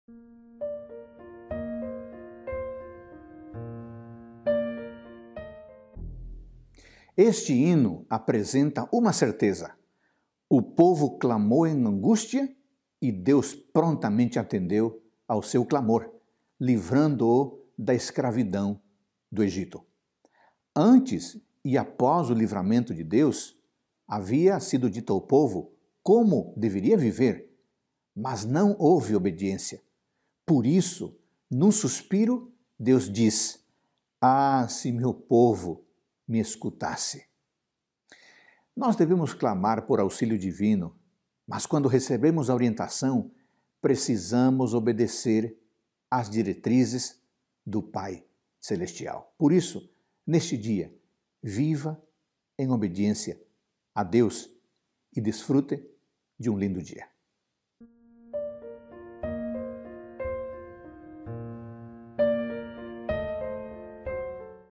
Salmos 81 - Reavivados por sua Palavra
Devemos clamar por auxílio divino, e quando recebermos orientação, precisamos obedecer às diretrizes divinas.